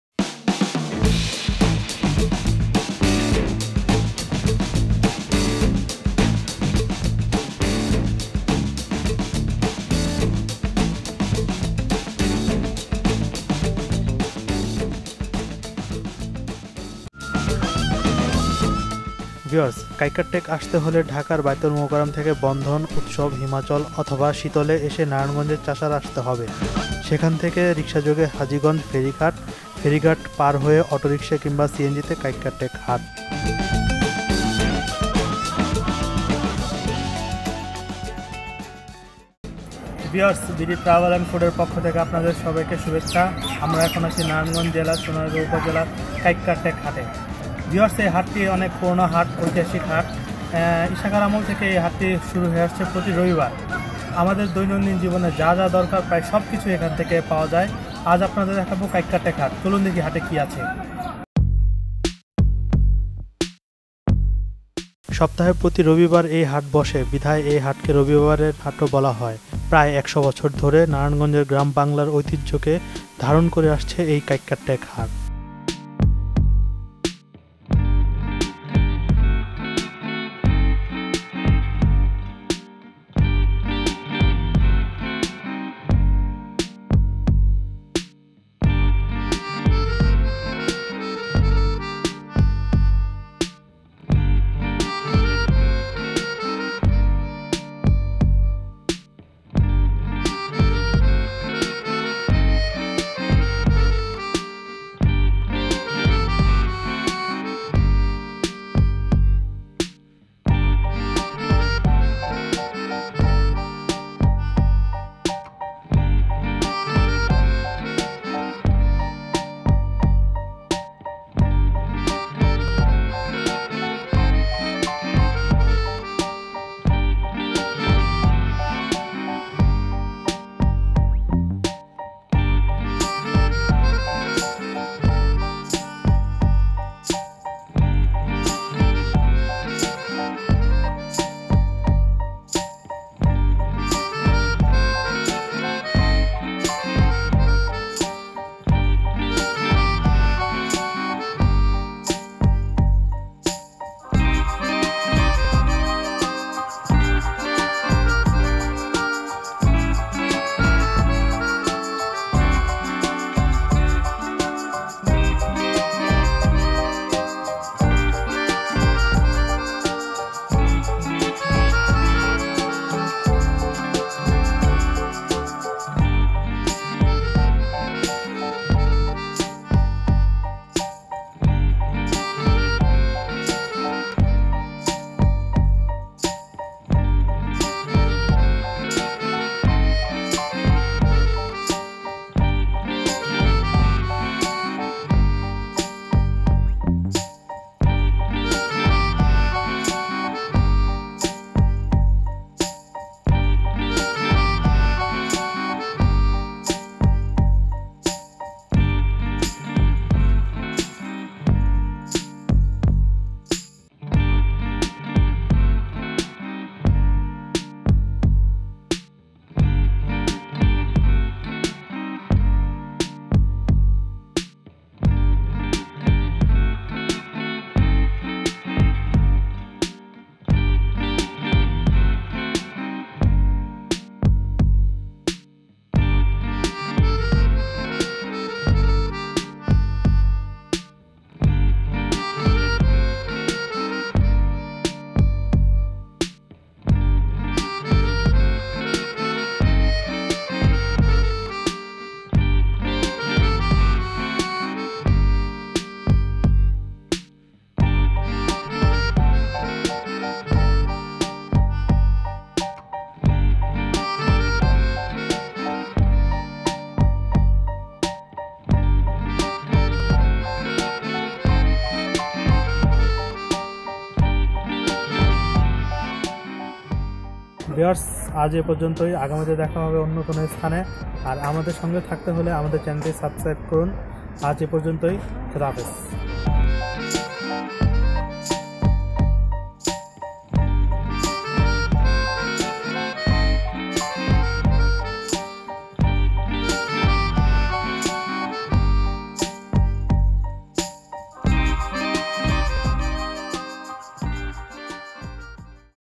ঐতিহাসিক কাইকারটেক হাট | KAIKERTEK HAAT
নারায়ণগঞ্জের সোনারগাঁ উপজেলায় শত বছরের পুরোনো  মোগরাপাড়া ইউনিয়নের কাইকারটেকে ব্রহ্মপুত্র নদের তীরে অবস্থিত ঐতিহ্যবাহী কাইকারটেক হাট । কোষা নৌকা, পোতা মিষ্টি, কাঠ, বাঁশ ও কৃষিজাত পণ্যের জন্য কাইকারটেক হাট নারায়ণগঞ্জ ও মুন্সিগঞ্জ জেলায় সুপরিচিত। গরু-ছাগল, হাঁস-মুরগি থেকে শুরু করে তরমুজ, চাল, ডালসহ কৃষকদের উৎপাদিত বিভিন্ন পণ্য পাইকারি ও খুচরা বিক্রি করা হয়। প্রতি রবিবার এই হাট হয়।